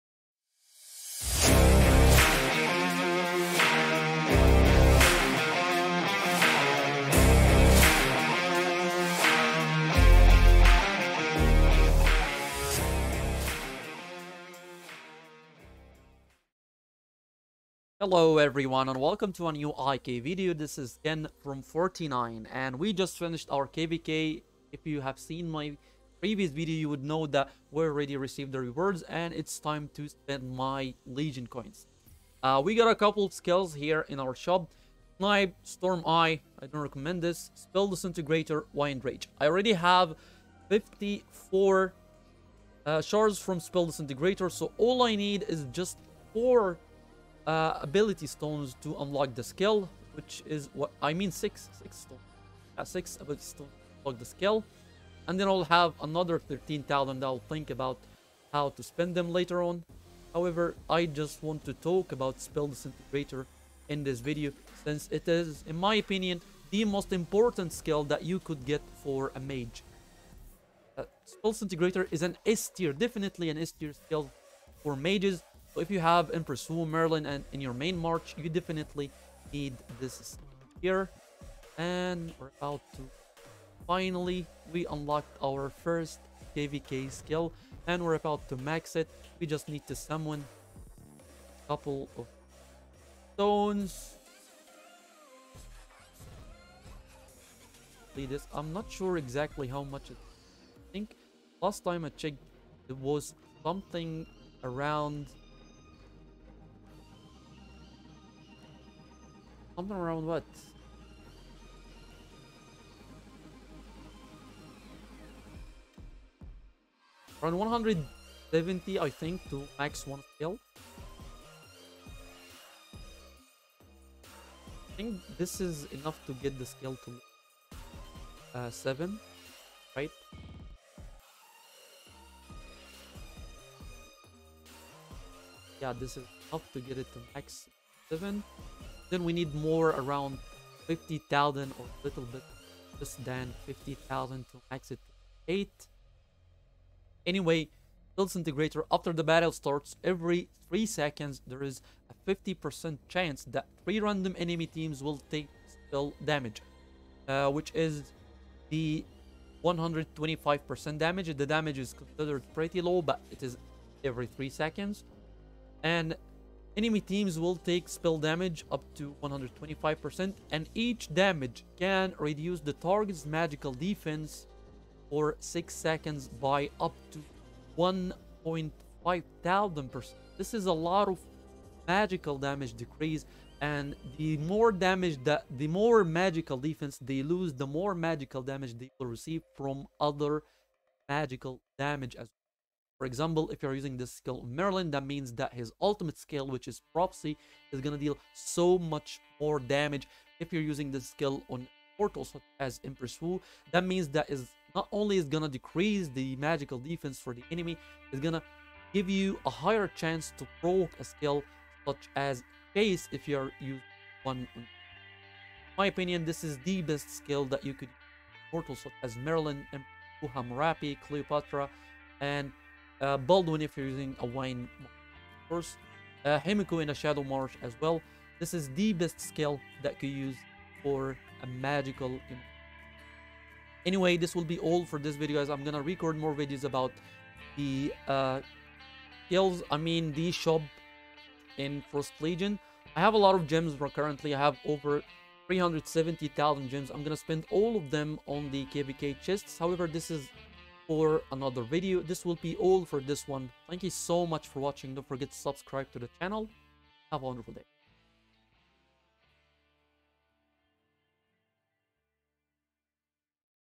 Unlocking my first KVK skill | Spell Disintegrator
Unlocking & Reviewing Spell Disintegrator

you wanna join 49? hit me up in discord!